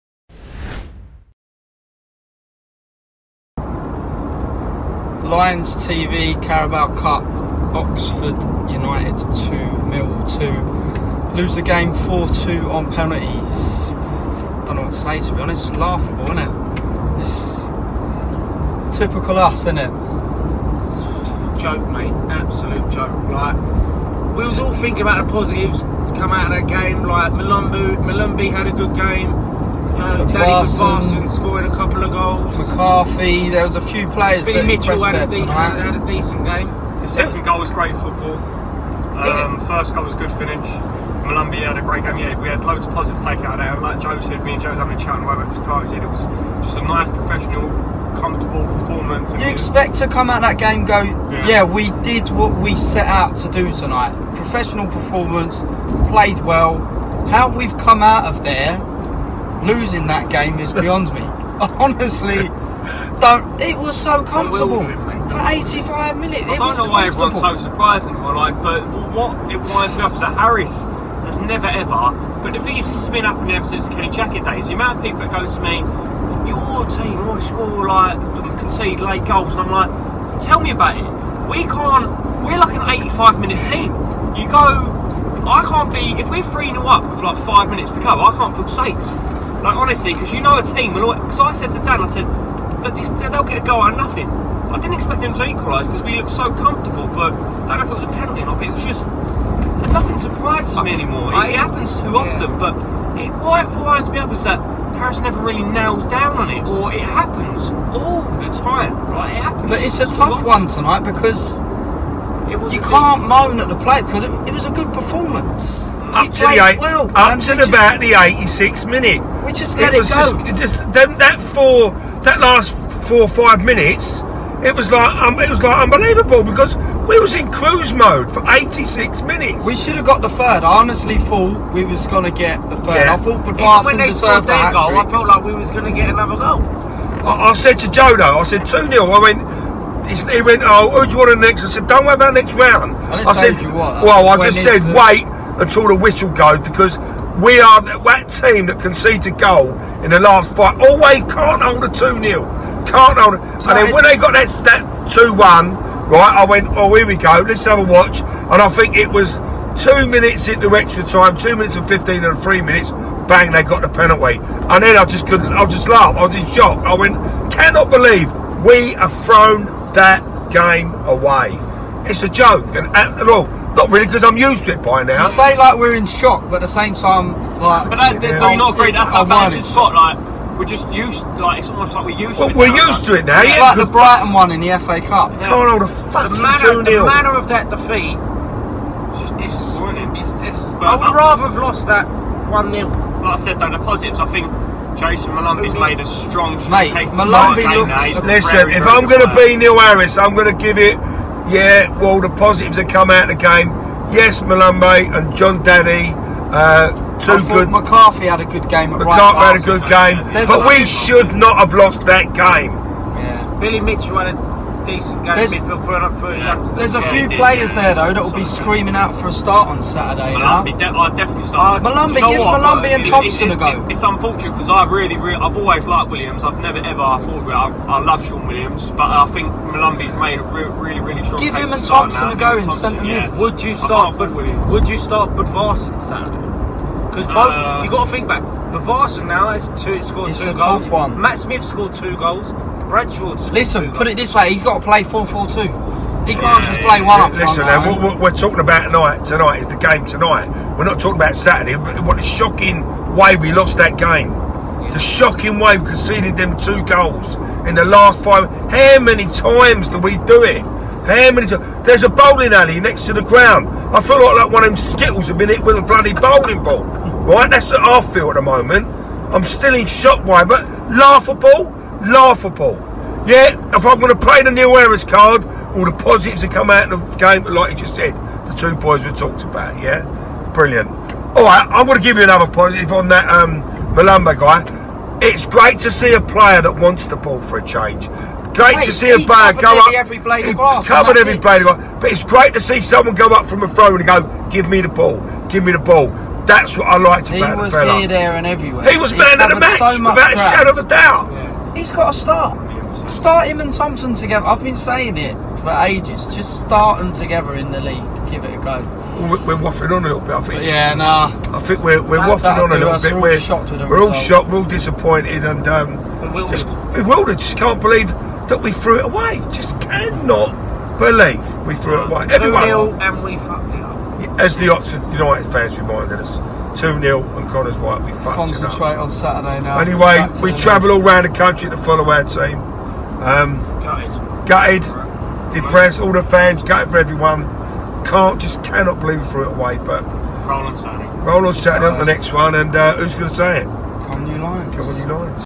FAN REACTION WITH THE O'MARAS- HOW HAVE WE LOST THAT GAME IT'S ACTUALLY LAUGHABLE
Fan reaction from tonight’s cup defeat away at Oxford.